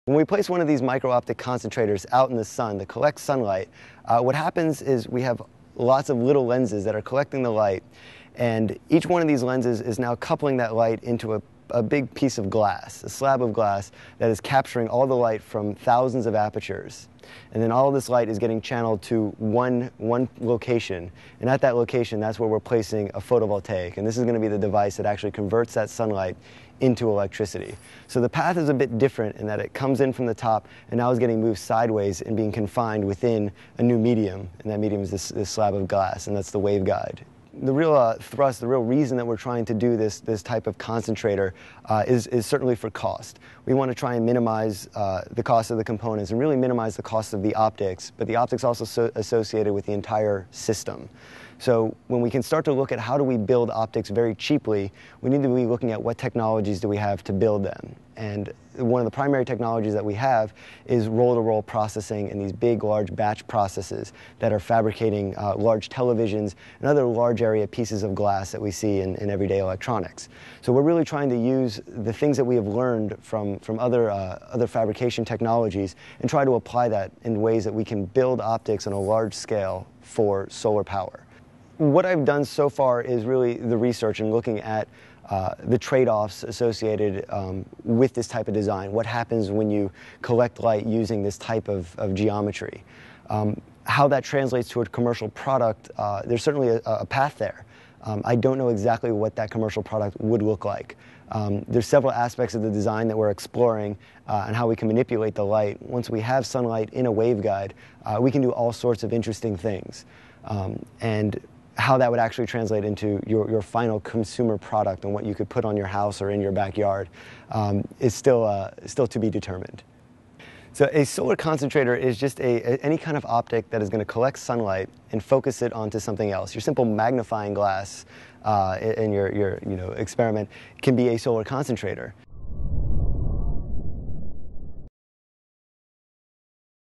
New Solar Concentrator Design from UC San Diego
A new solar concentrator design from an electrical engineering PhD student at the University of California, San Diego could lead to solar concentrators that are less expensive and require fewer photovoltaic cells than existing solar concentrators.
The graduate student, Jason Karp and his colleagues presented the design in a paper in the January 2010 issue of the journal Optics Express On April 15, Karp and his solar concentrator won the 2010 Rudee Research Expo Outstanding Poster Award at the 29th Annual Research Expo at the UC San Diego Jacobs School of Engineering